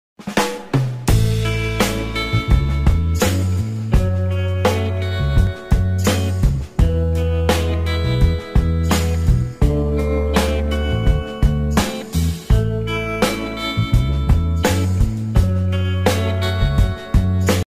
【miyanailstudio】魔镜粉水滴彩绘美甲光疗甲nail
1刷一层清透色
2用封层用笔画出水滴状
3魔镜粉涂抹亮面
4封层完成